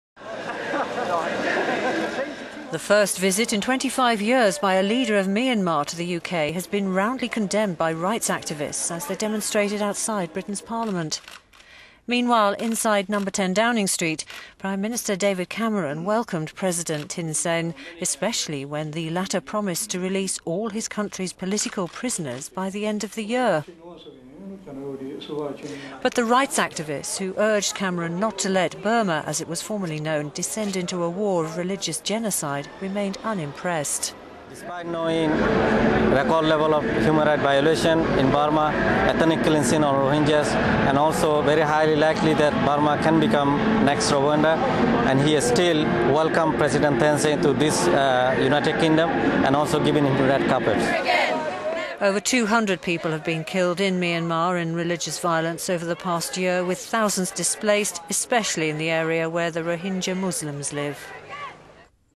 Burmese rights activists sceptical over president's pledge to free political prisoners
The first visit in 25 years by a leader of Myanmar to the UK has been roundly condemned by rights...

The first visit in 25 years by a leader of Myanmar to the UK has been roundly condemned by rights activists as they demonstrated outside Britain's parliament.

Meanwhile inside Number 10 Downing Street, Prime Minister David Cameron welcomed President Thein Sein, especially when the latter promised to release all his country's political prisoners by the year's end.

But the rights activists who urged Cameron not to let Burma - as it was formally known - descend into a war of religious genocide, remained unimpressed

Ahamed Jarmal of the Burmese Rohingya organisation said:

"Despite knowing record level of human right violations in Burma, ethnic cleansing of Rohingyas and also that it is very likely Burma could become next Rwanda, and he still [David Cameron] welcomes President Thein Sein to this United Kingdom and gives him the red carpet."